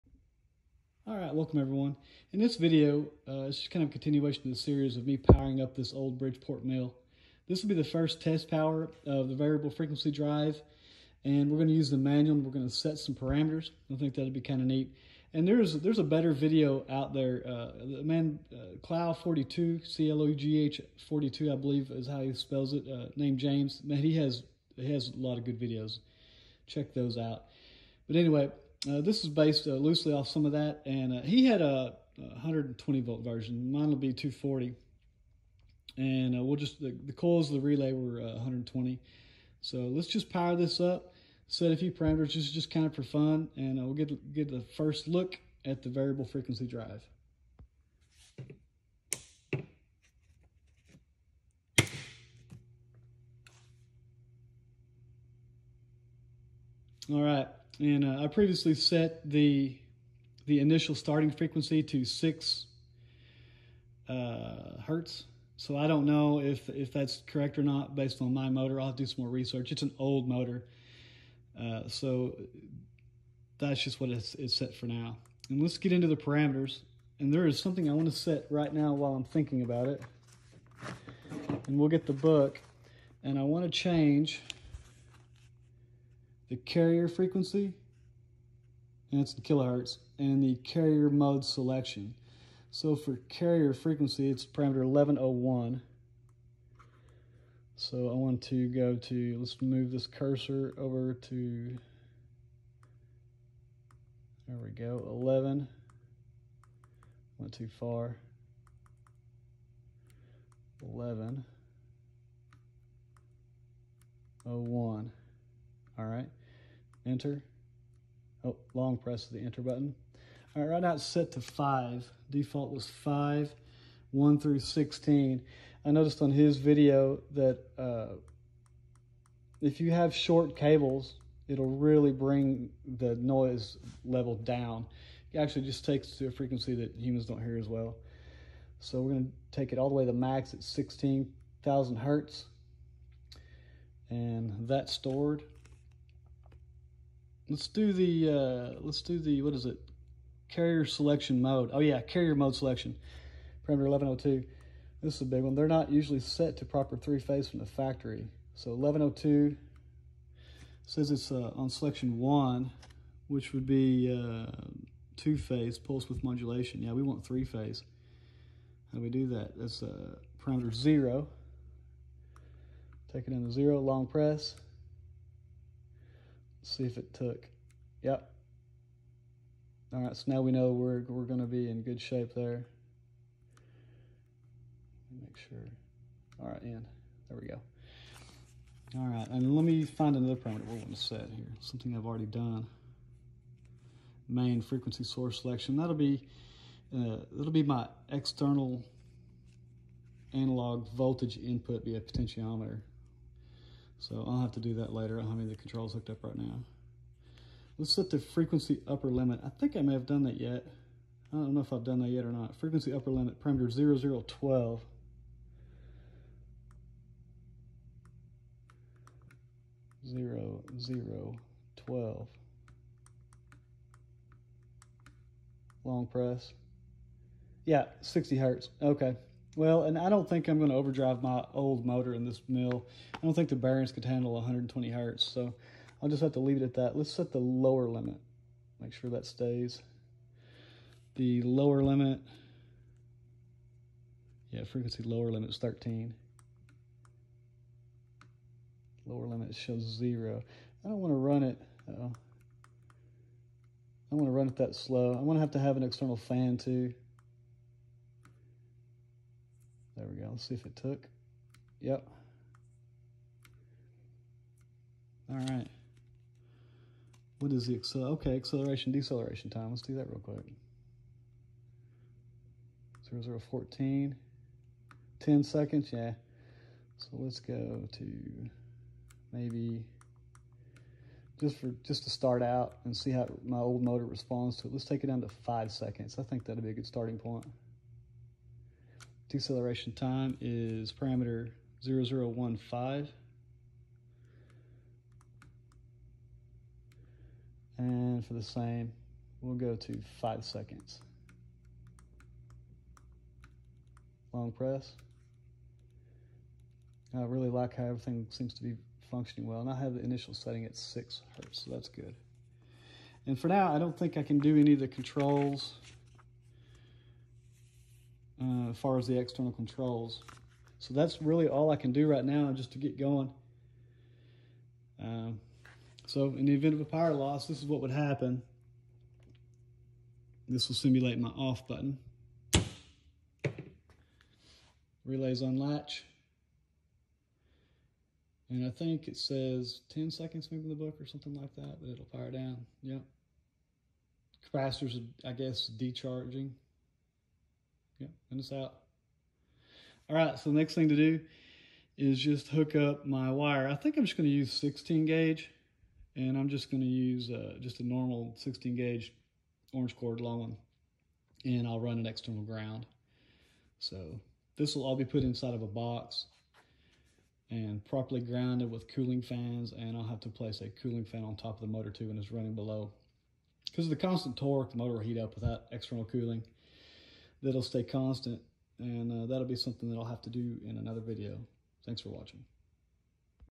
Milling machine power Part 3 Testing the VFD and setting some parameters
A quick video of a test power up of the VFD for the Bridgeport milling machine.